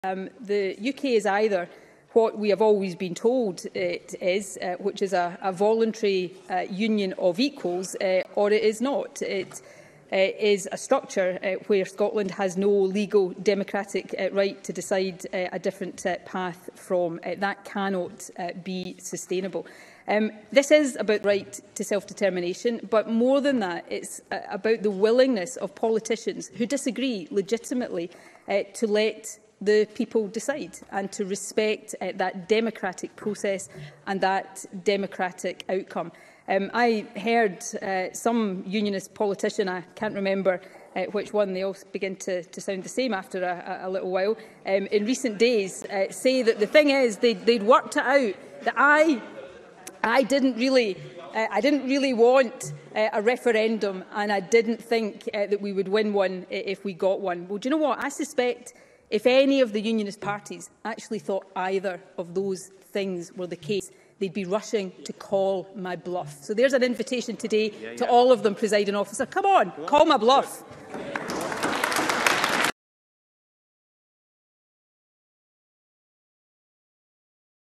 Watch Nicola Sturgeon's brilliant response to claims that she doesn't really want independence
The First Minister was speaking after she gave a statement setting out the 'route map' to indyref2.